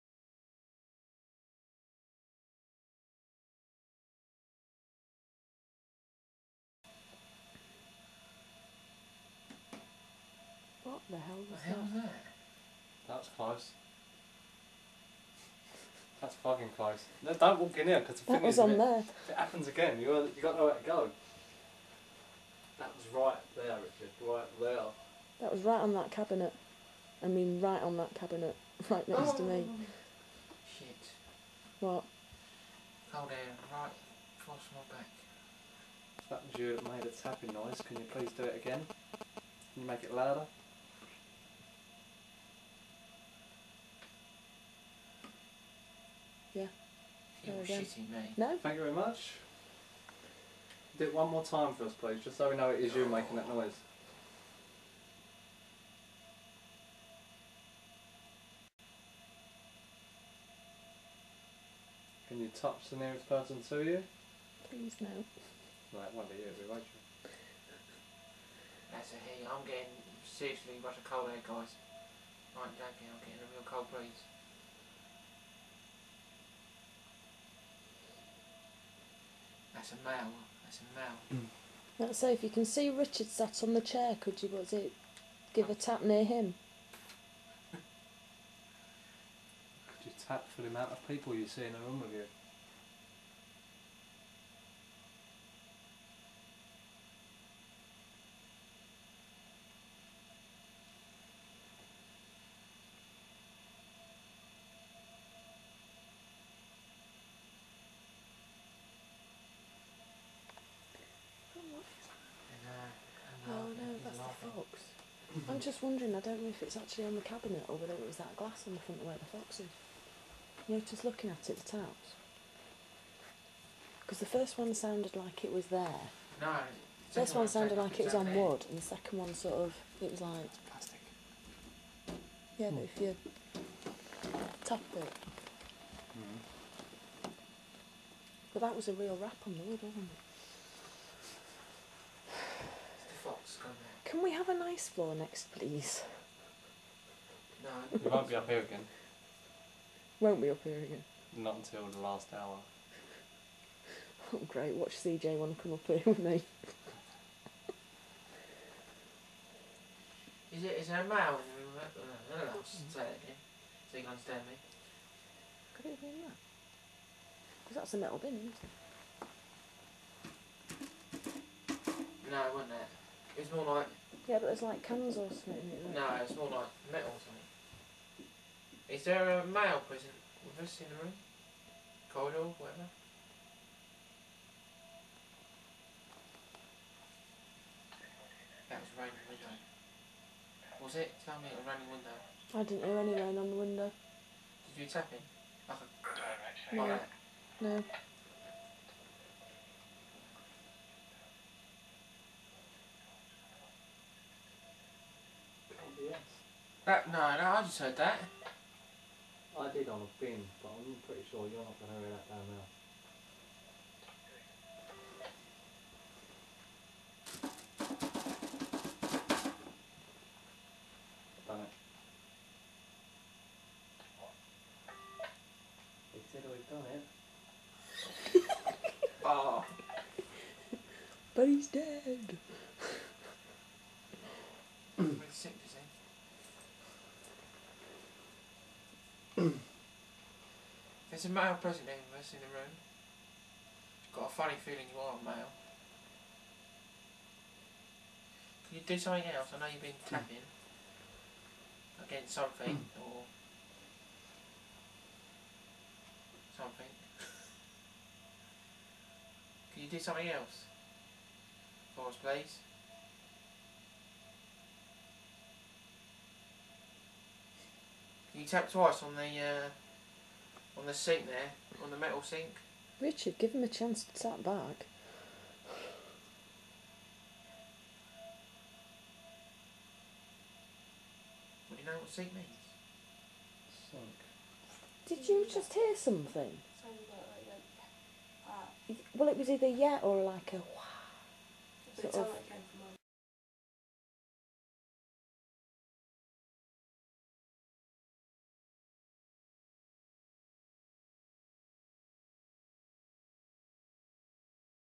Clitheroe Castle Museum
Paranormal Investigation at Clitheroe castle and museum